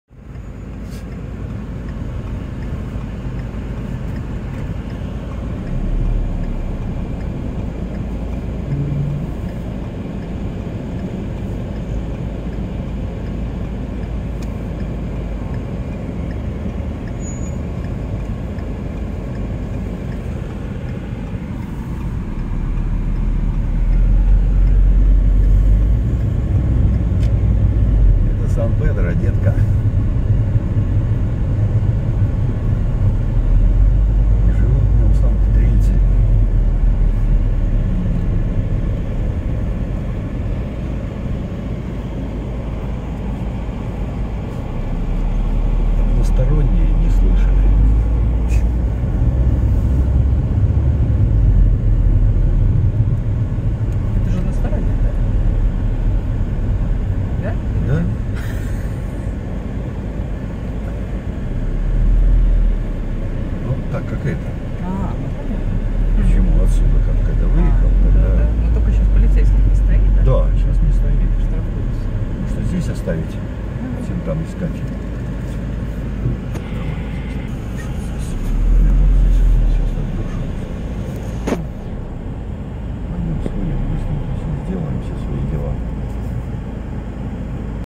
Сан Педро
История любви и правда о Доминикане.